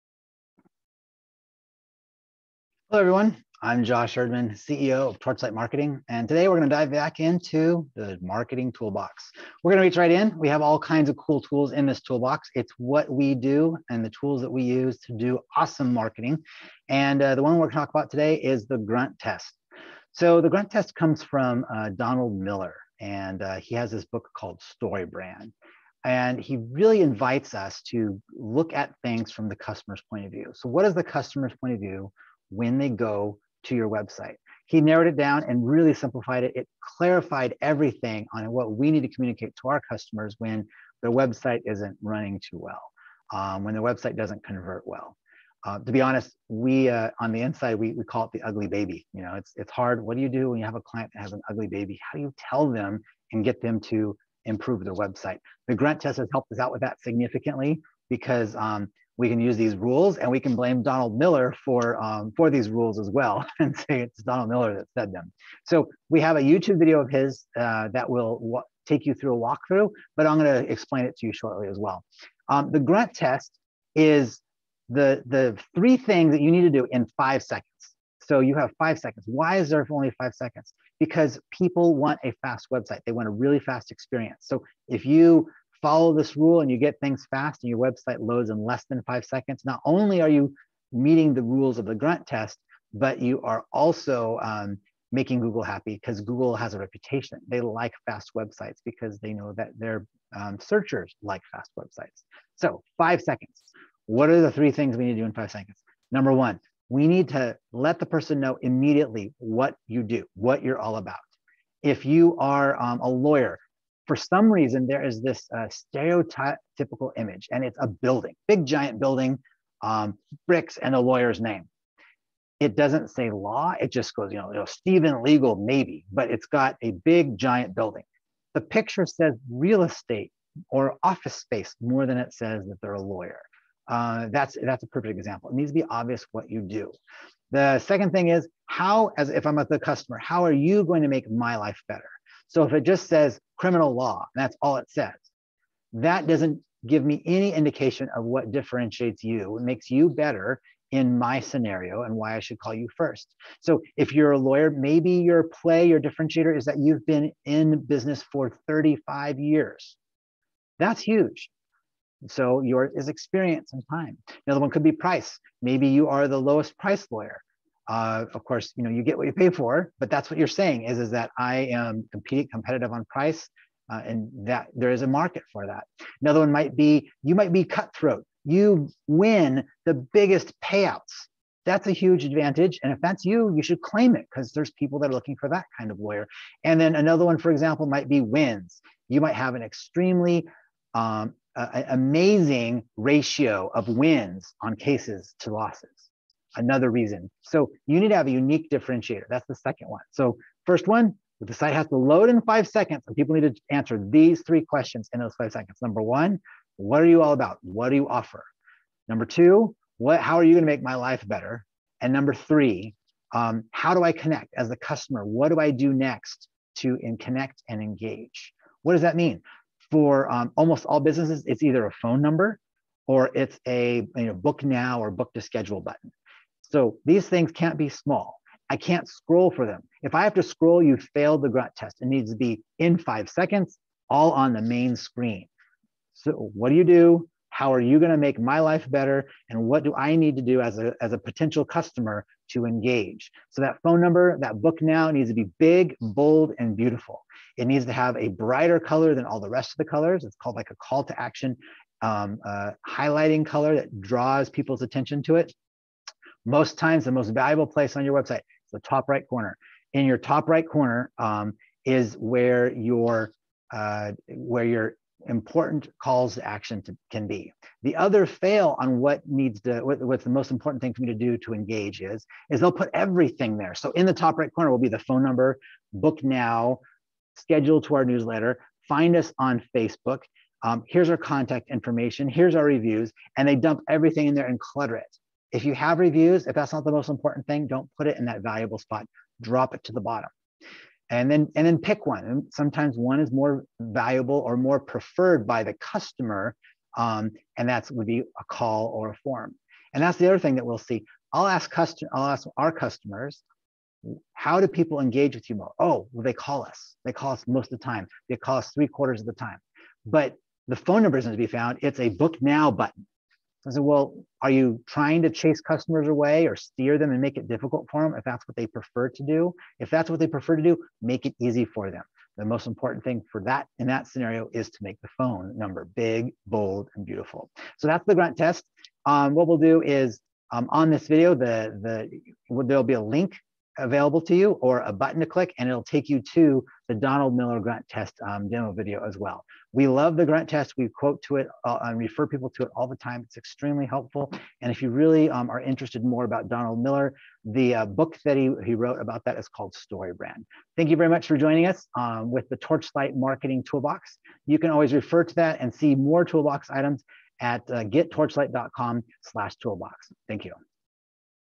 TLM's Marketing Toolbox: The Grunt Test
Next up we have a personal favorite of the TLM team, the Grunt Test!

The Grunt Test, created by Donald Miller, is a technique used to determine how easily a new user would view your site. The three questions you must be able to answer within the first 30 seconds of visiting your site (and without scrolling!) are:
1. What do you offer?
2. How will you make my life better?
3. How do I connect, buy, move forward with purchase, etc.?

If you cannot answer all three questions within 30 seconds and without scrolling any further down, your homepage might need some adjustments.